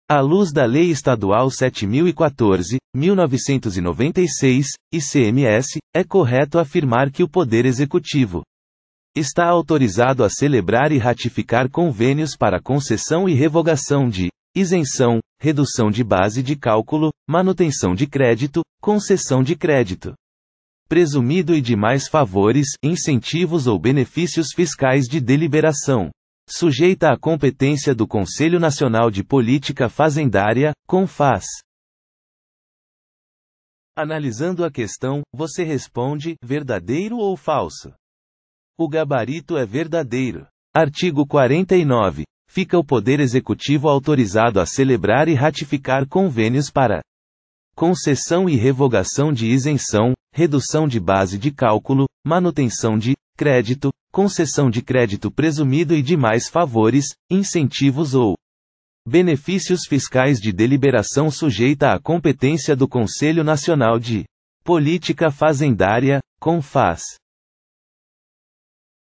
QScon Direito: [Art. 49, Lei Estadual 7.014/96 - BA] Texto de Lei
Art. 49, Lei Estadual 7.014/96 BA | Texto de Lei

À luz da Lei Estadual 7.014/1996 (ICMS), é correto afirmar que o Poder Executivo. está autorizado a celebrar e ratificar convênios para concessão e revogação de. isenção, redução de base de cálculo, manutenção de crédito, concessão de crédito. presumido e demais favores, incentivos ou benefícios fiscais de deliberação. sujeita à competência do Conselho Nacional de Política Fazendária (CONFAZ).

Gabarito: Verdadeiro